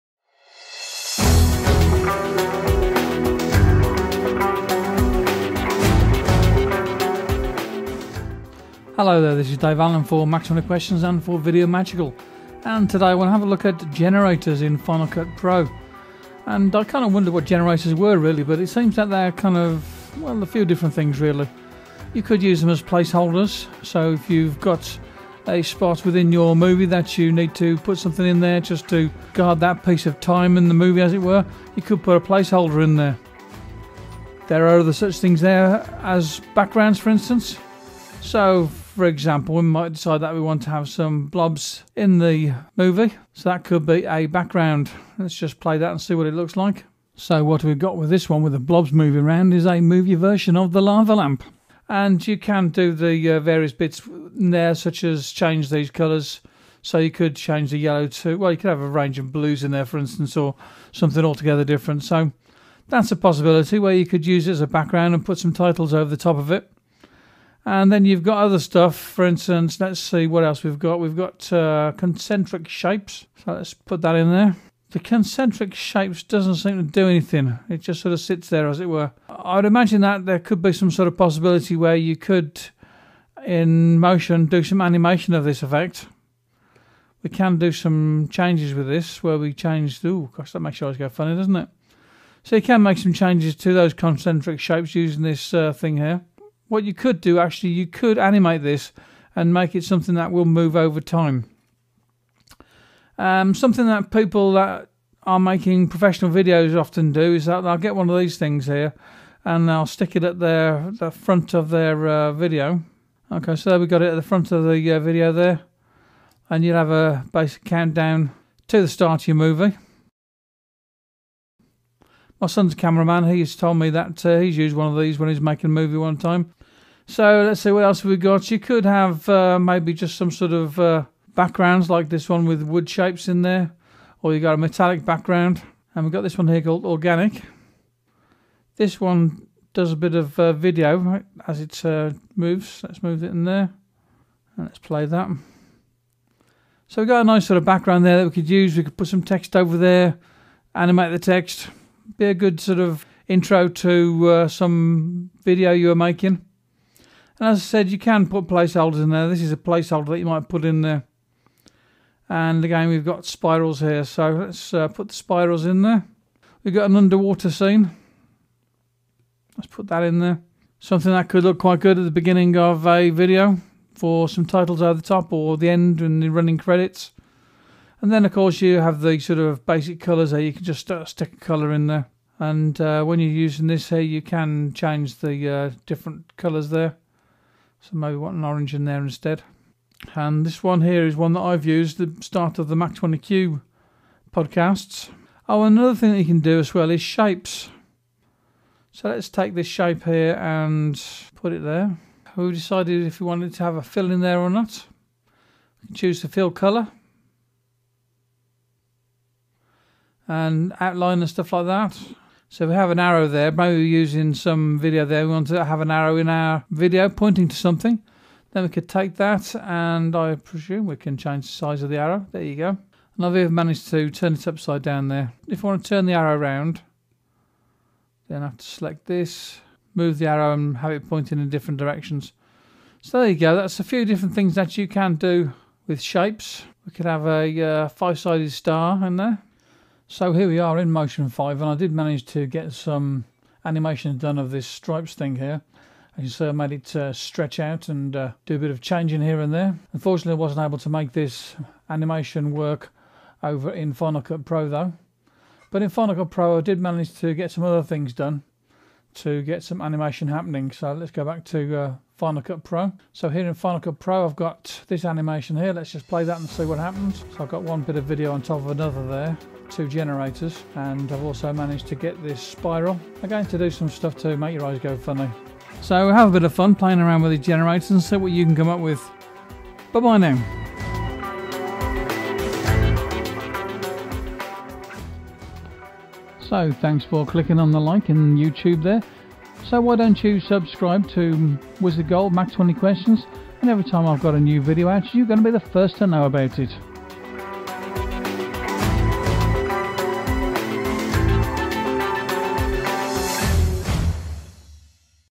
Generators in FCPX and Motion 5
I kind of wondered what generators were in FInal CUt Pro and it is kind of that they are shapes, video clips, placeholders and some of them are still and some of them move. You can use them as backgrounds for other parts of your video, such as using the one that is like you are looking at a sort of under water type of scene or even the flat colours. You can put titles over them or composite the generator to get other effects happening.

There is a generator to put a time code on the screen, and another that does a countdown at the start of the video, bot of these more likely to be used by professionals in the movie video world.

There here are clips that are textures to be used in various artistic ways. On top of that you might use a generator as a place holder for a video clip that has not yet been shot. Use it to fill in a space and let you know how it might work with the real shot in there later.

I was able to get animations happening with the key framing of published parameters of generators like the concentric circles and the stripes. I did try to do the animation in Motion 5 but then when I used it in FCPX it just stayed still doing nothing. I must have missed a step along the way or something. I will get to the bottom of the problem.